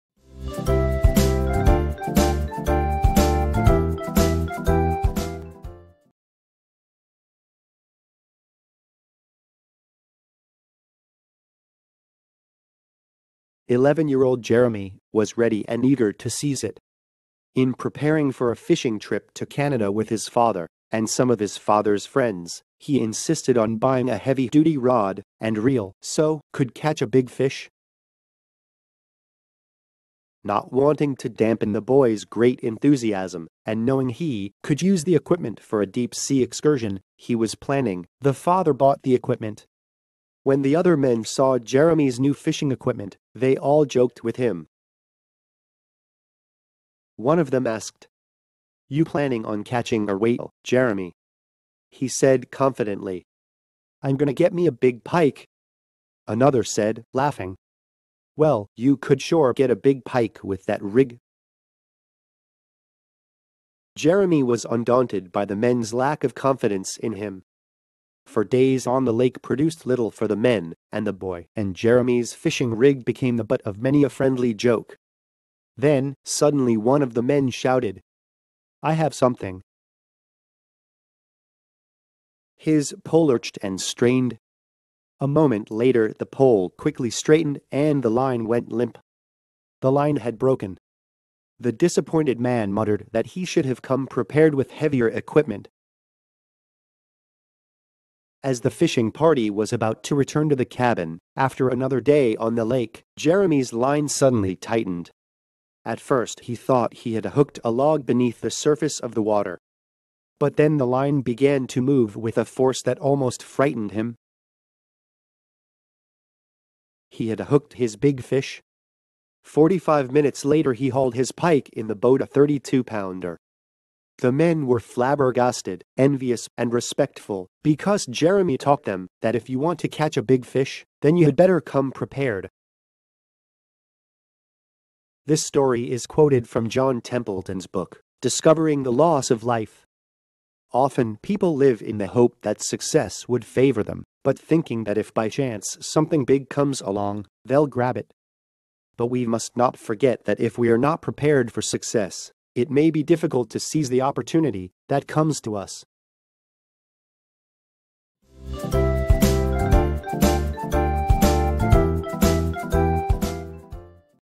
Short Story: The reason why a boy was able to catch a big pike
This story is quoted from John Templeton's book [Discovering the Laws of Life]. Often people live in the hope that success would favor them, but thinking that if by chance something big comes along, they’ll grab it. But we must not forget that if we are not prepared for success, it may be difficult to seize the opportunity that comes to us.